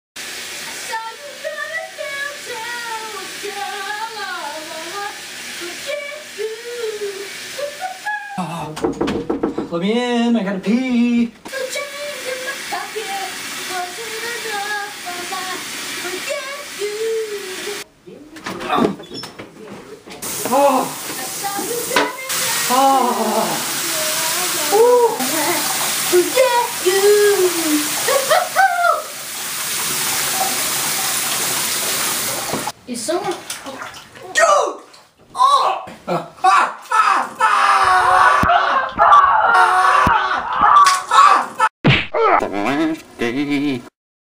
Pee
This is why you should never sneak up on people in the bathroom.